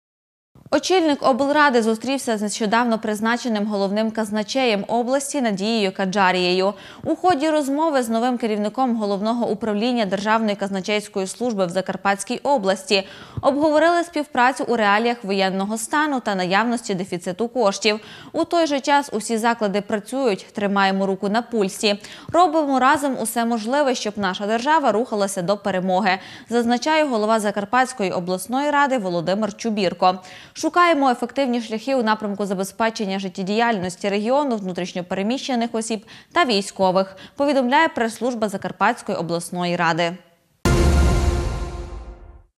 Казначейство працює
Очільник облради зустрівся із нещодавно призначеним головним «казначеєм» області Надією Конджарією. У ході розмови з новим керівником Головного управління Державної казначейської служби у Закарпатській області обговорили співпрацю у реаліях військового стану за наявності дефіциту коштів.